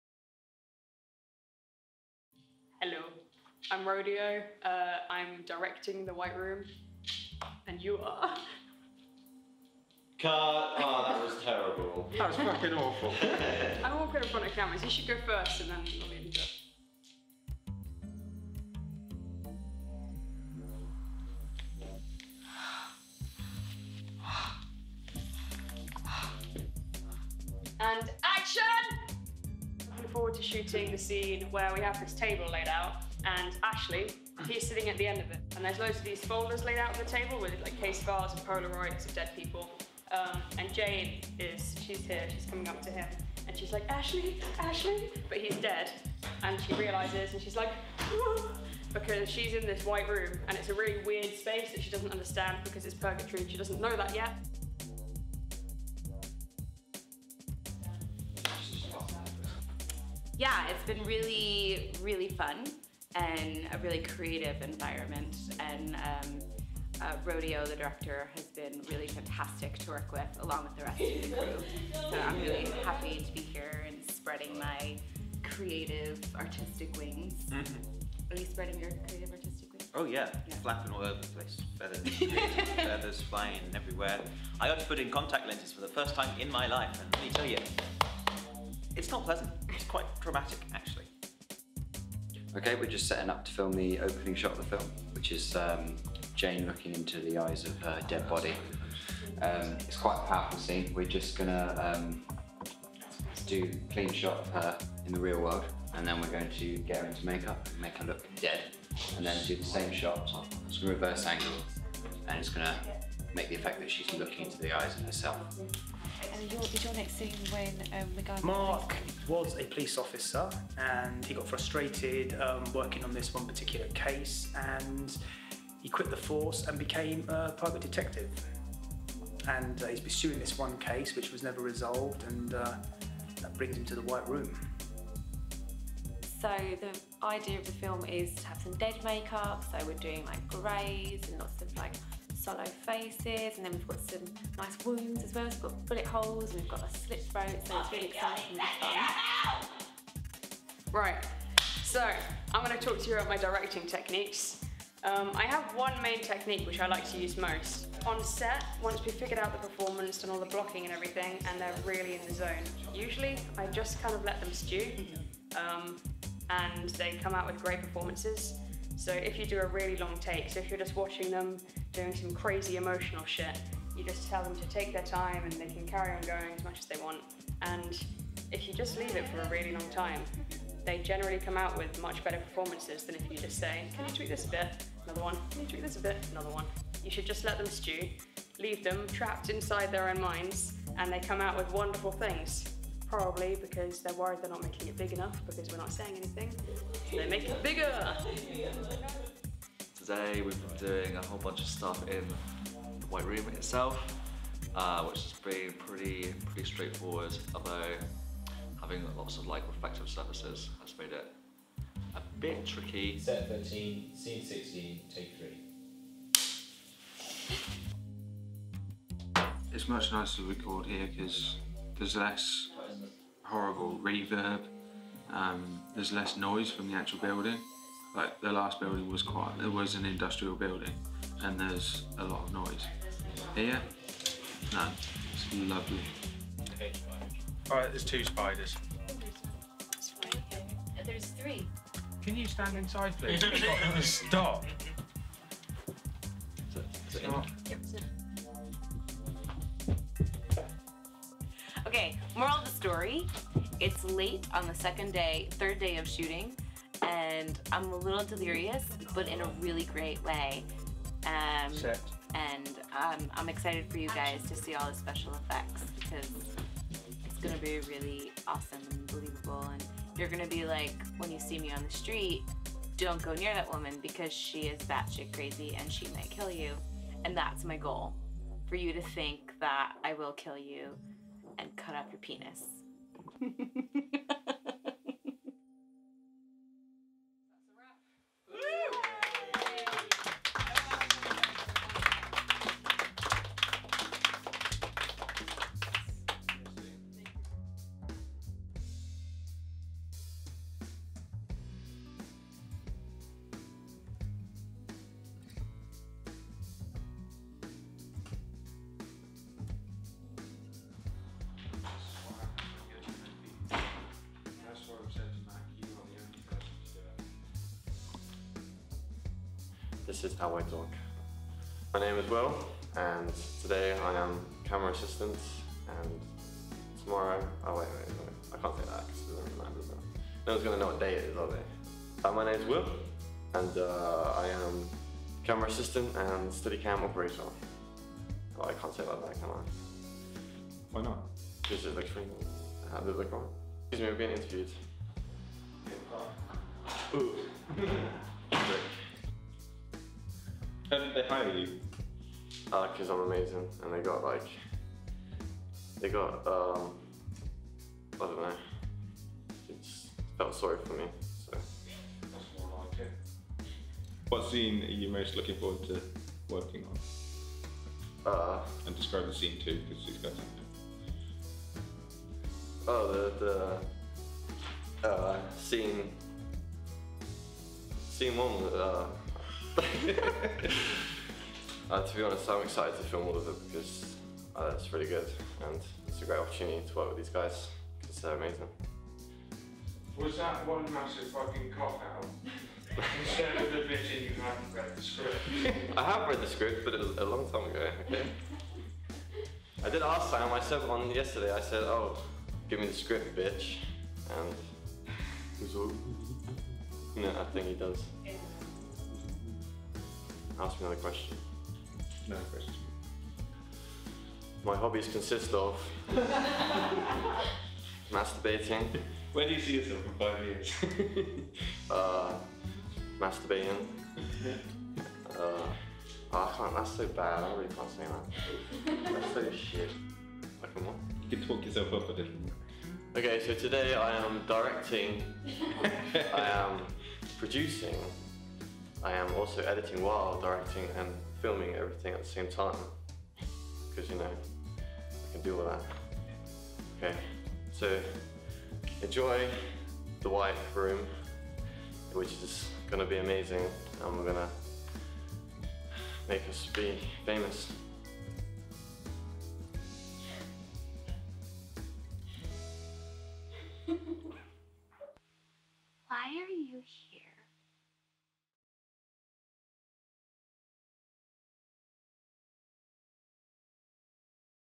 The White Room | Behind The Scenes
Ever wonder what goes on behind the scenes of a Rodeax shoot?
The White Room was shot over 3 days in Oxfordshire, 2016.

STARRING: Candice Palladino, Robin Kirwan, David East

Behind The Scenes video shot by Sam Scholfield
Edited by Rosanna Strange
Audio by Daniel Strange

Jane awakens to a void.

Trapped in a blank white world, she is taunted by strange and horrific visions. Fact or fantasy, she isn't sure.

Only when the strings of fragmented memories begin to knot together, does she realise where she is and why.